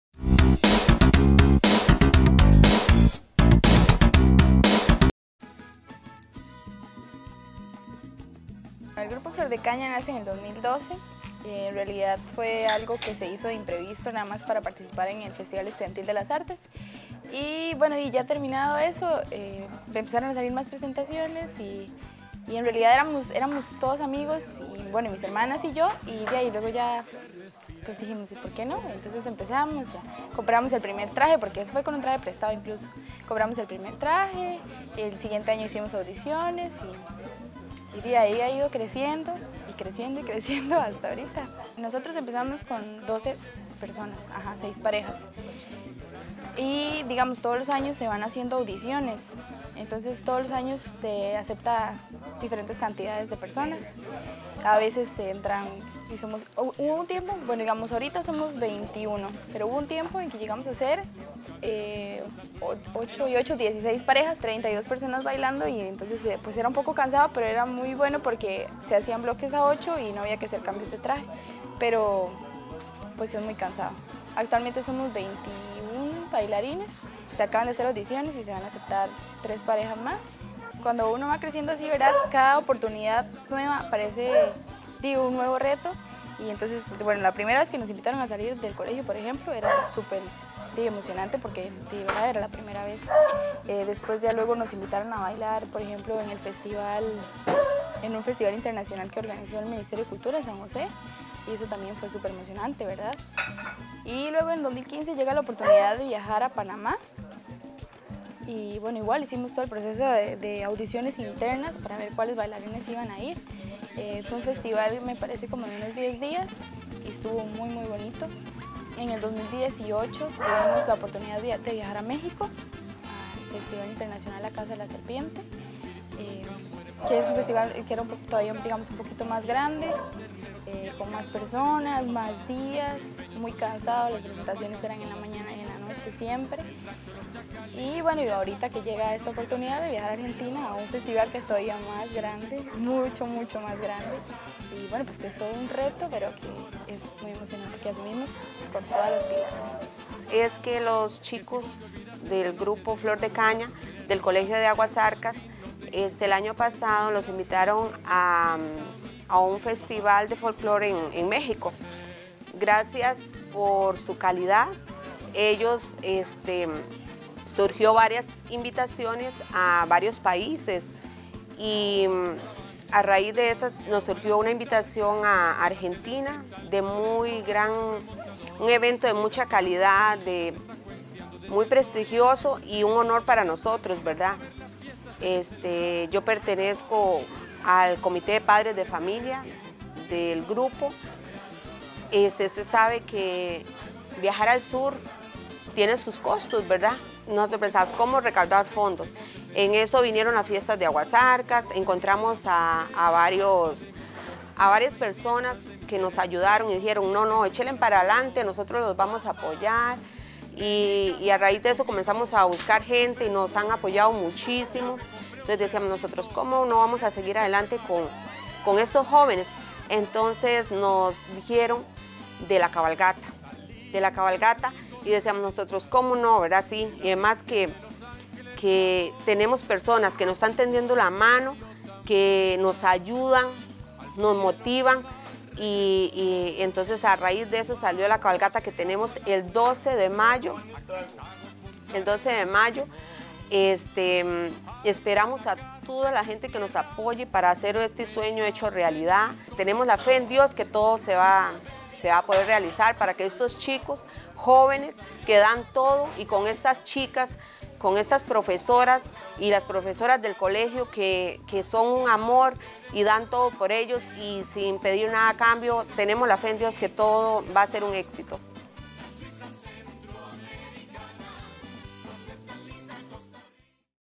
Grupo Flor de Caña
Grupo Folclórico del CTP de Aguas Zarcas participará en Festival mundial a realizarse en Argentina.